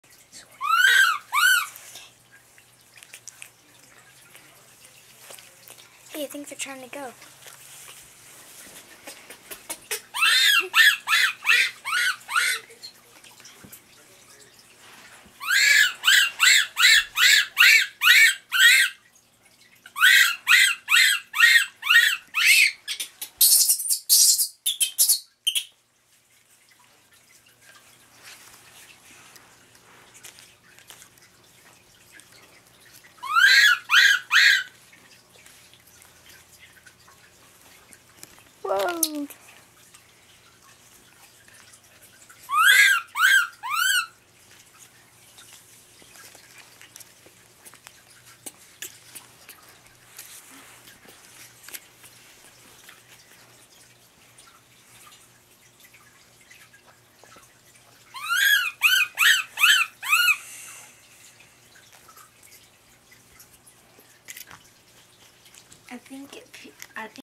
Monkeys at Pic"a"Pal
Two baby rhesus macaque monkeys that are 4 weeks old at Pic"a"Pal Pet store.